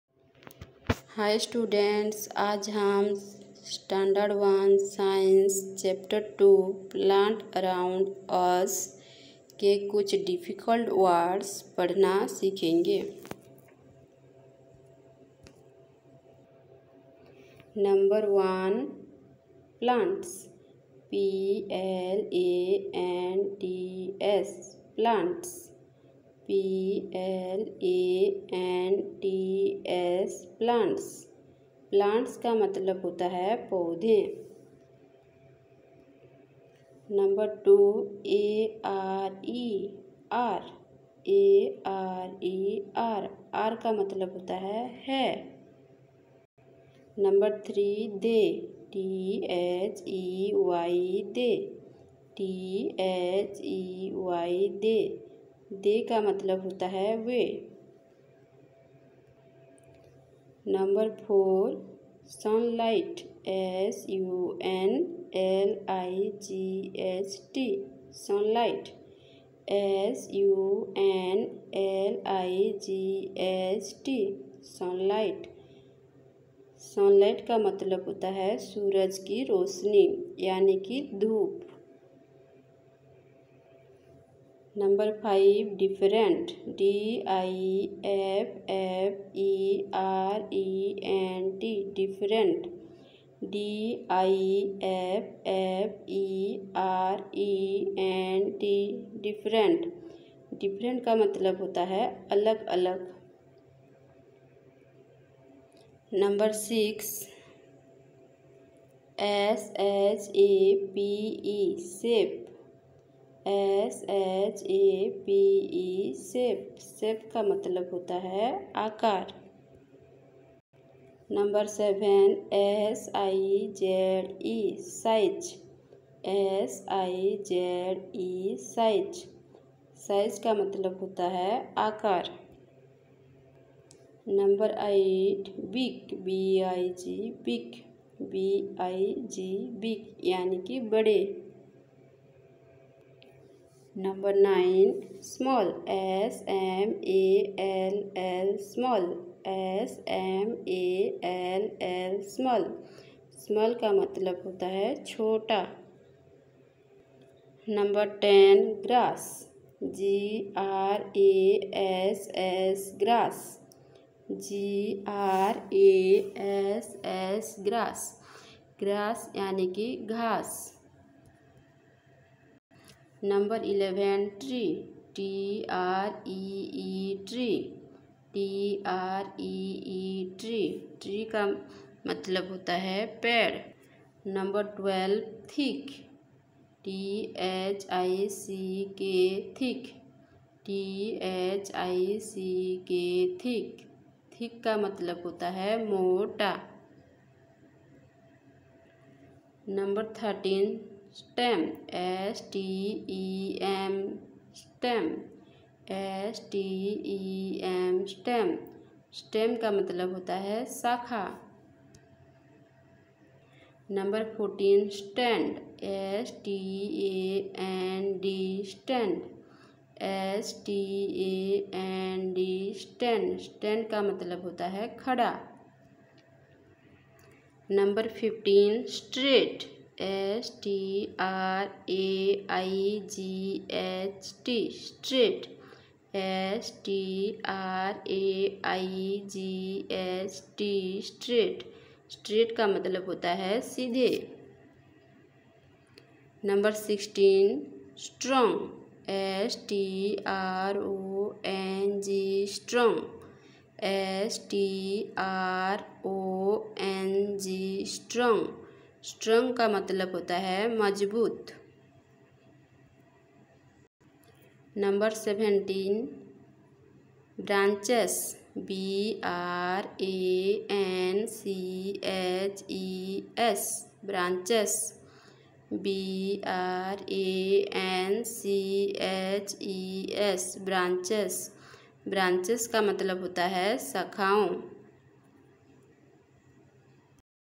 STD.1- SCIENCE- CHAPTER 2 - PLANTS AROUND US ( learning difficult words part.1)
holy faith abc of science, class.1
chapter.2 - plants around us